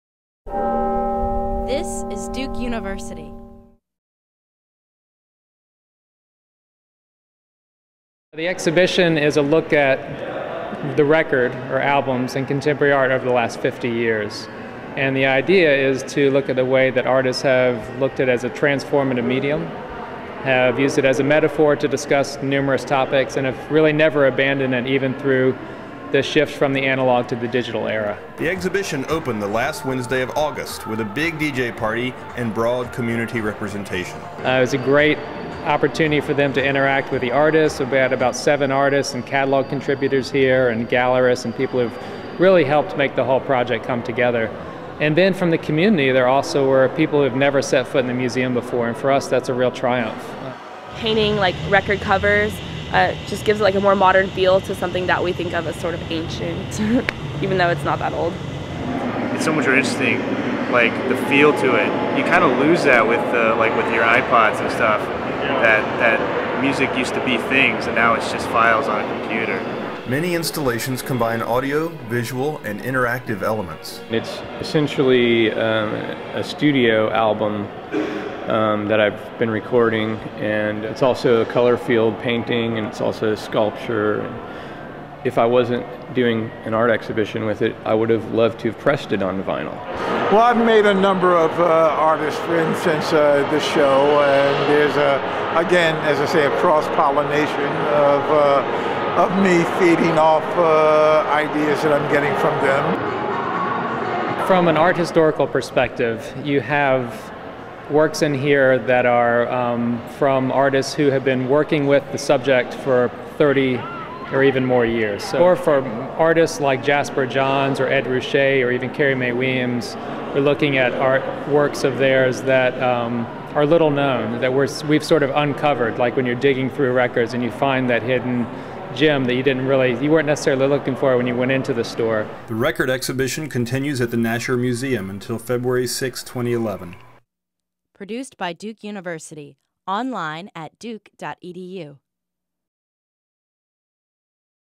The Record: Contemporary Art and Vinyl
At Duke University, a new Nasher Museum of Art exhibition celebrates the culture of vinyl records.